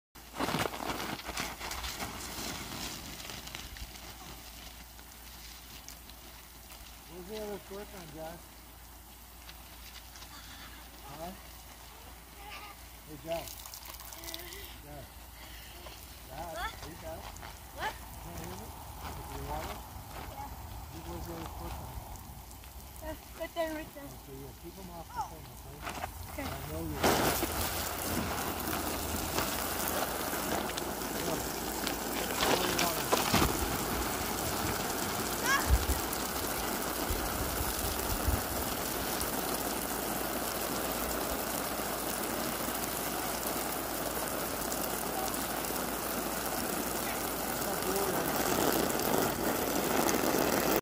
my kids having fun slip and sliding Grandpa's wedding them so they can slide down part 1/3
my kids having fun at slip and slide where we live